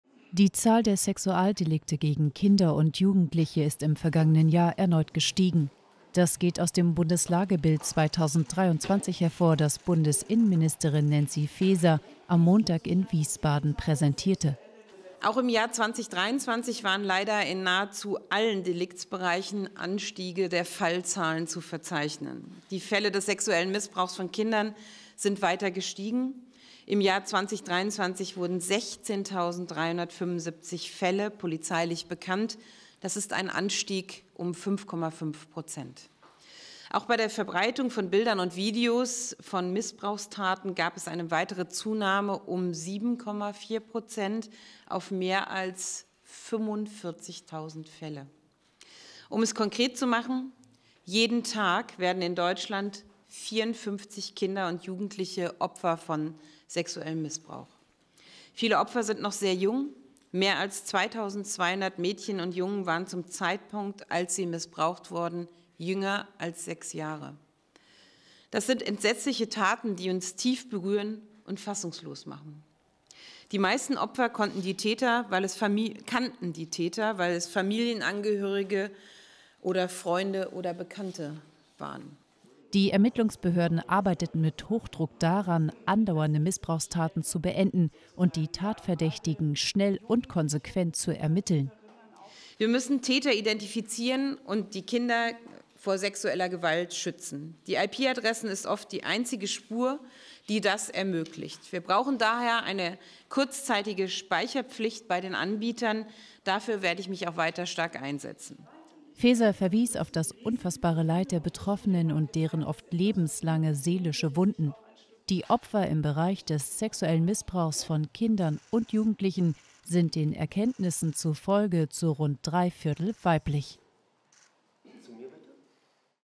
Immer mehr Sexualdelikte gegen Kinder registriert
Im vergangenen Jahr sind die Fallzahlen von Sexualdelikten gegen Kinder laut BKA erneut gestiegen. Jeden Tag werden demnach 54 Kinder Opfer sexuellen Missbrauchs.

Bildrechte Thumbnail: Neil Hall/Reuters